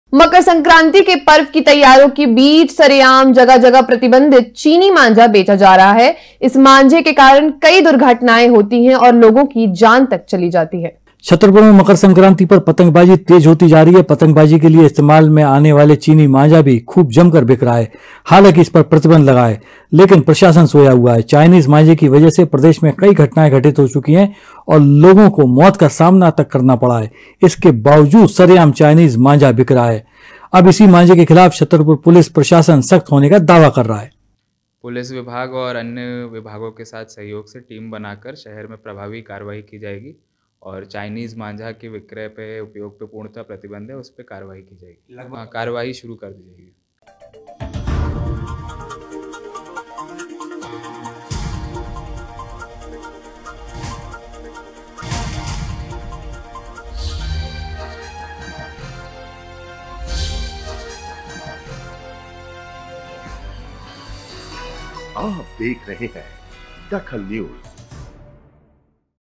DAKHAL NEWS लोगो की जान लेता है चीनी मांझा
लोगो की जान लेता है चीनी मांझा
प्रतिबंध के बावजूद बिक रहा है